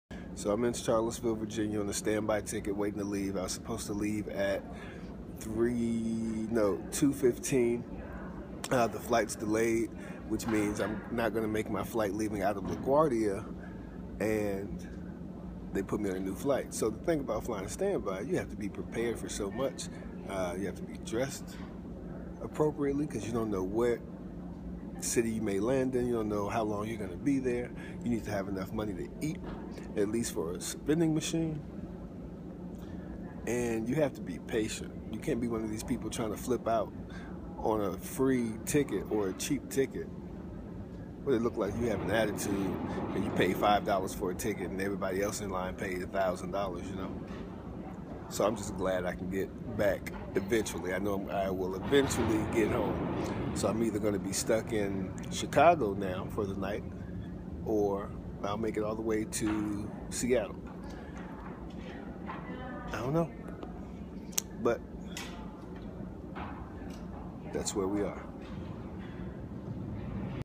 Flying Standby
The Thing About Flying Standby.

Patience, as the cliché goes, is a virtue. Get to the airport an hour or so before the very first flight headed toward your destination, and be prepared to be at the airport all day, hoping and wishing.

Things to know:

1. Show up knowing all possible alternative routes.

2. DO NOT get an attitude with representatives unless they get an attitude first.

3. Dress like you’re going to a religious temple and FLIRT like you’re in a hoe house. This may get you into that open first class seat they couldn’t sell.

4. Carry your necessities in your carry on just in case they send your checked luggage days before they send you.

5. Have enough money to eat at least 3 regular meals. Airport food is not cheap usually.

6. If you must, keep calling the customer service line. They all don’t follow the book. Someone will eventually help you.

7. Call ahead to find out the possibility of you getting on the flight. Ask what number you are on the list.